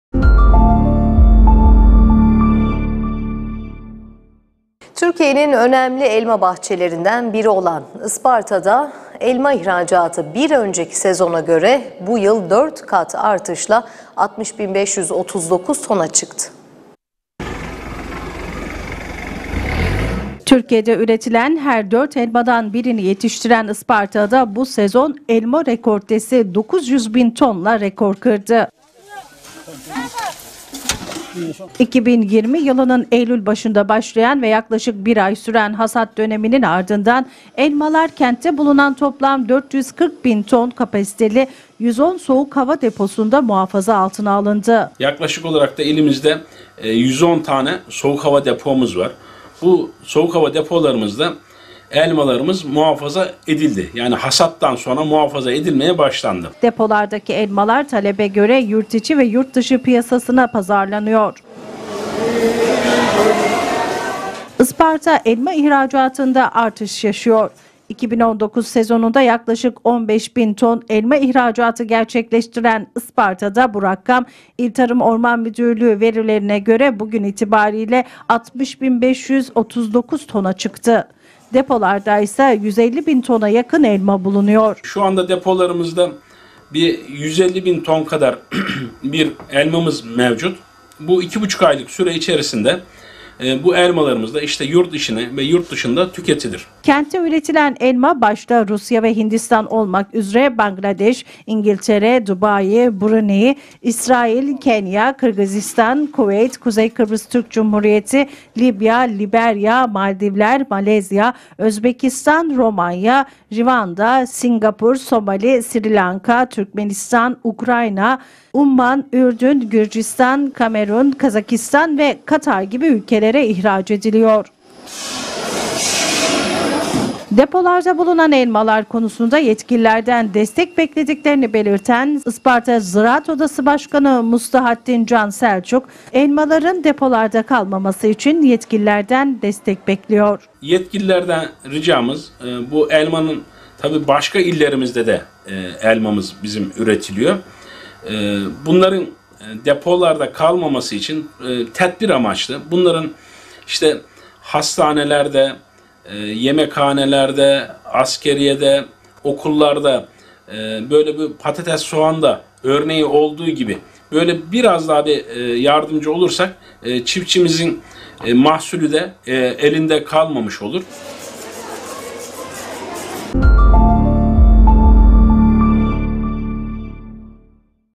2019’da 15 Bin Ton Olan Elma İhracatı Bu Sezon 60 Bin Tona Çıktı
TÜRKİYE’NİN ÖNEMLİ ELMA BAHÇELERİNDEN BİRİ OLAN ISPARTA’DA ELMA İHRACATI BİR ÖNCEKİ SEZONA GÖRE BU YIL 4 KAT ARTIŞLA 60 BİN 539 TONA ÇIKTI.